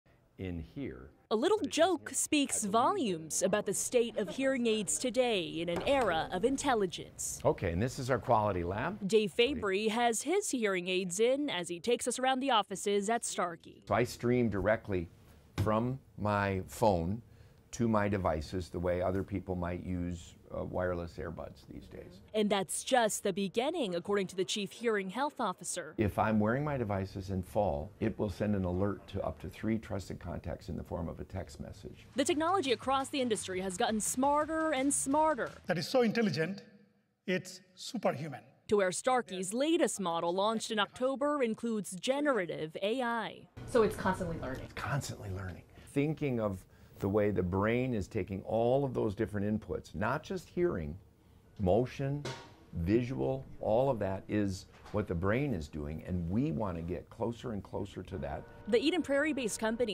ai hearing aids changing the game see how Starkey’s smart tech in action
One in six adults has hearing loss, and AI is transforming how we hear.